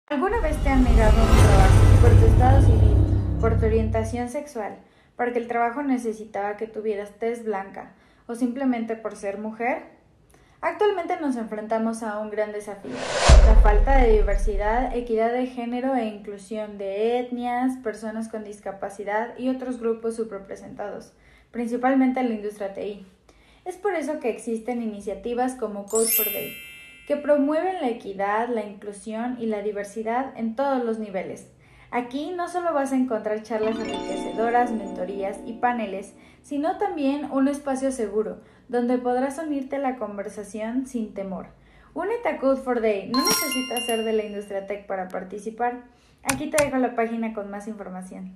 Code 4 DEI | El software grandioso es diverso, equitativo e incluyente.
Code 4 DEI es una iniciativa que busca promover la inclusión y el desarrollo en todos los niveles de la industria tecnológica, concientizando a quienes no pertenecen a grupos vulnerables y eliminar barreras para alcanzar todo su potencial y fomentar la innovación.